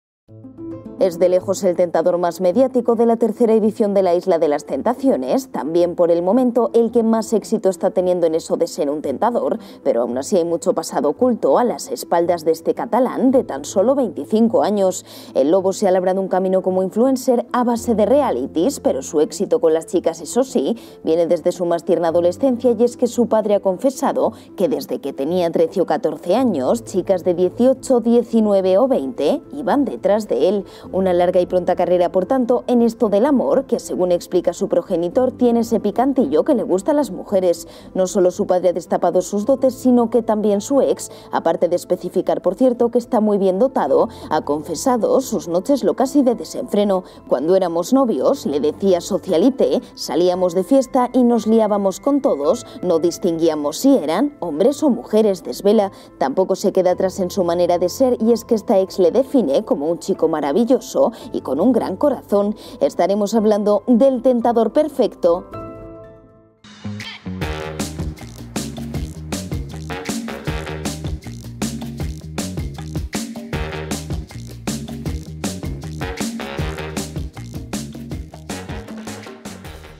¡Nueva filtración! Tras el vídeo de Marina e Isaac sale a la luz esto de La isla de las tentaciones
El vídeo de Marina e Isaac ha puesto en el punto de mira a estos dos participantes de La isla de las tentaciones 3. Algo que ha llevado ahora a que se destape el pasado oculto de Isaac tamabién conocido como El Lobo.